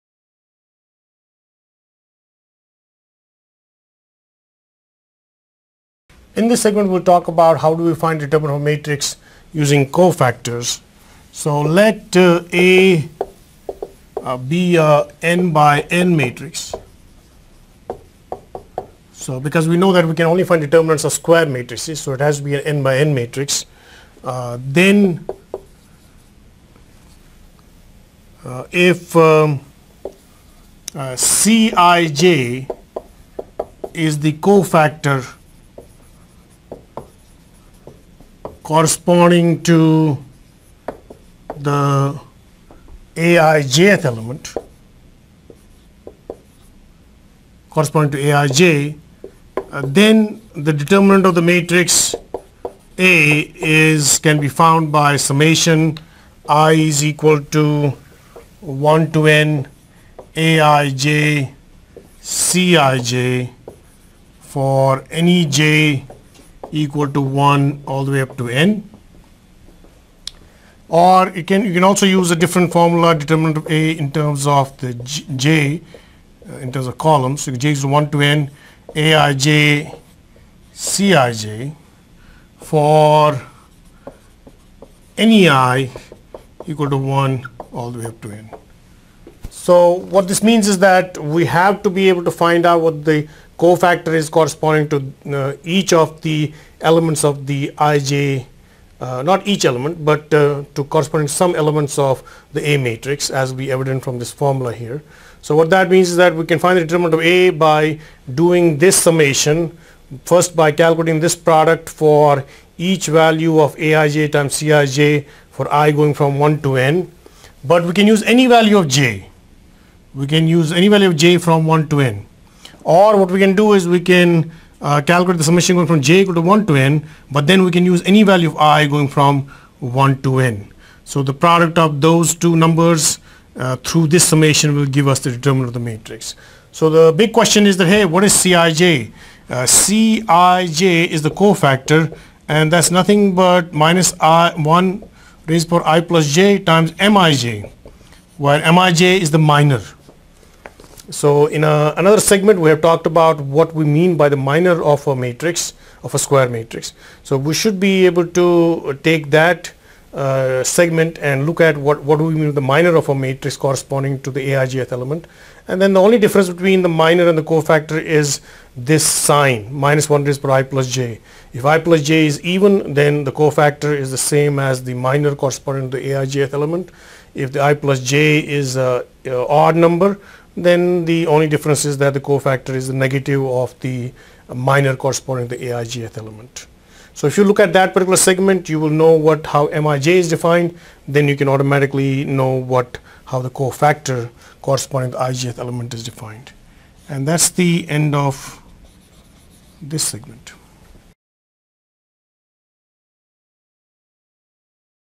Chapter 04.04: Lesson: Determinant of a Matrix Using Cofactors: Theory
Learn the theory of determinant of a matrix using cofactors. For more videos and resources on this topic, please